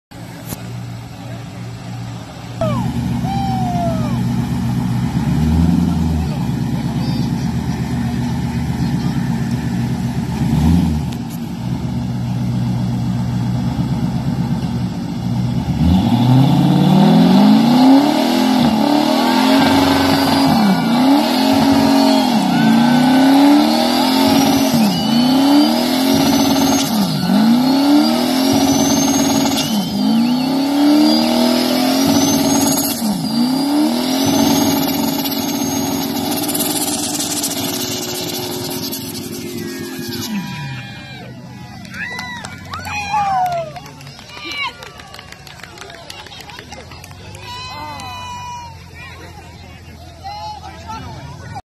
Burnout Gender Reveal || ViralHog
Occurred on August 25, 2018 / Delmar, Iowa, USA

"This was a gender reveal we did for our son, Kamdyn, due January 2019. After losing our infant son, Kyler, in 2017, we were devastated and didn’t know if we could go through the pregnancy process again. When we did find out we were pregnant again, our car club family, QC Gearheads, helped set this reveal up. In the video, two good friends are in my Monte Carlo doing a burnout with special color smoke tires. Those two where the only two who knew what the gender was until the blue smoke rolled. It’s a boy. This video captures our reactions so perfectly."